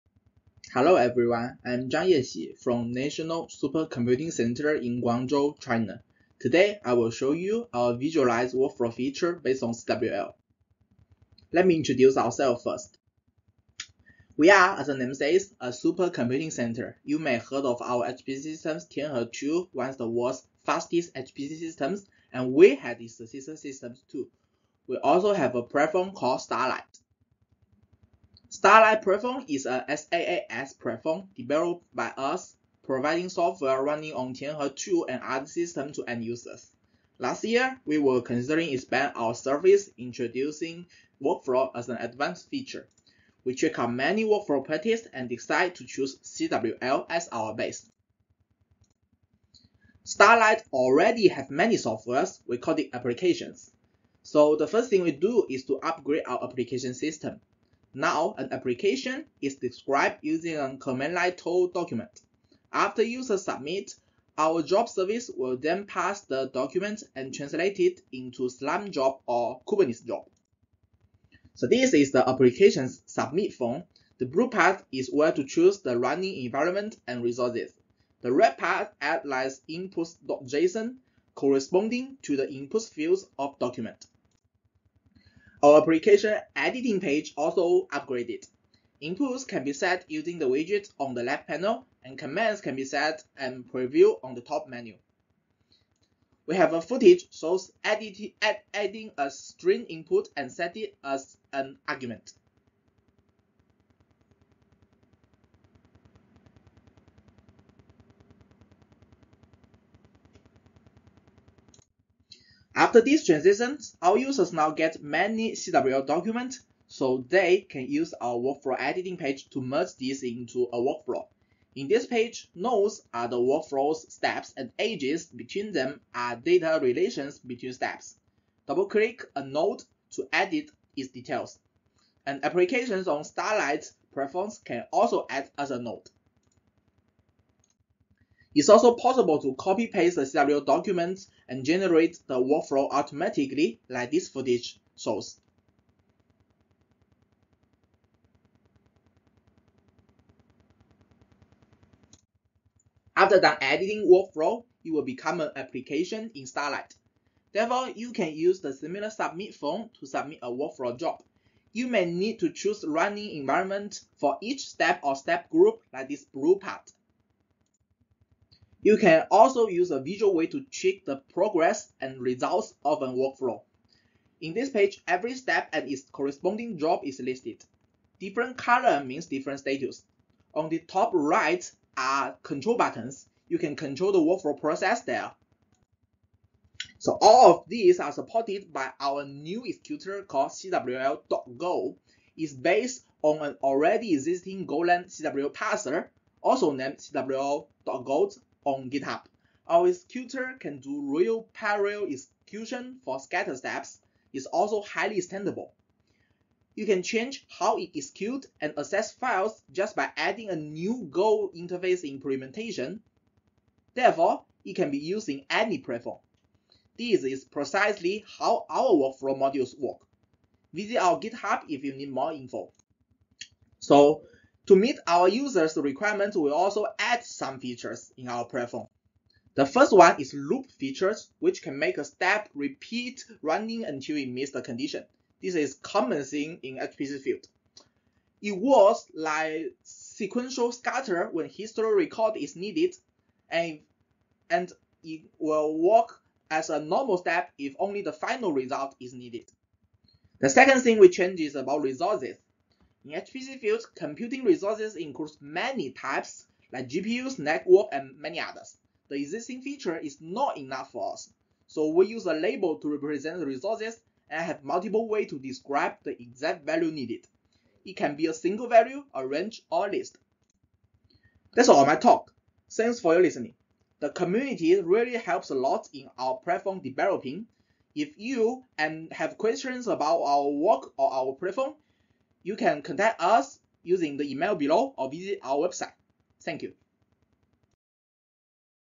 Extending CWL for High-Performance Computing: A Visual Workflow System with HPC Enhancements
Presenter: Zhang Yexi, Starlight Develop Team, National Supercomputer Center in Guangzhou, China

Summary: In this presentation, we delve into our cutting-edge development: a visualization workflow editor and submission system based on the Common Workflow Language (CWL), with a special focus on our extensions tailored for High-Performance Computing (HPC) environments. Our primary enhancements revolve around loops and resource descriptions, designed to significantly streamline the creation and management of complex workflows. These adaptations enable researchers and engineers to utilize computational resources with greater efficiency. Through an intuitive drag-and-drop interface and advanced workflow configuration options, users can effortlessly construct, test, and deploy workflows optimized for HPC settings. We will demonstrate how these CWL extensions empower users to leverage HPC-specific capabilities, such as batch job management and parallel computing, thereby enhancing the execution efficiency of computational tasks. Join us to explore how this innovative tool can propel the forefront of scientific research and engineering projects by optimizing workflow management for HPC environments.